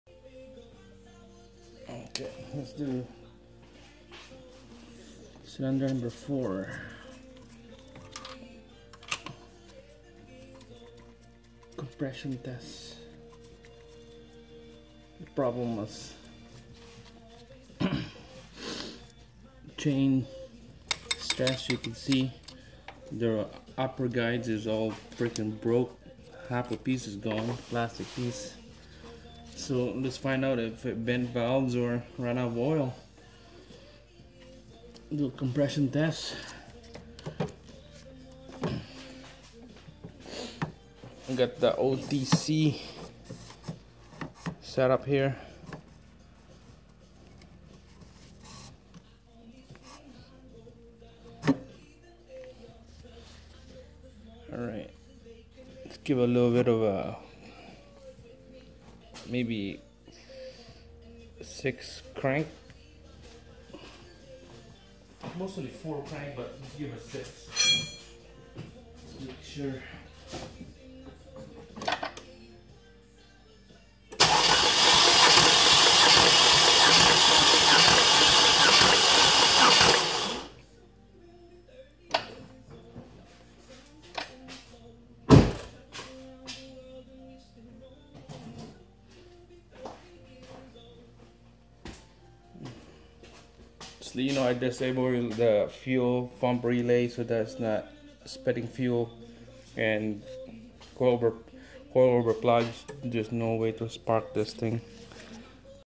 How I Compression Cylinder Test Chev 2008-14 Malibu 2.4L ecotect
Oct compression test.  Over 100 psi is  good cylinder..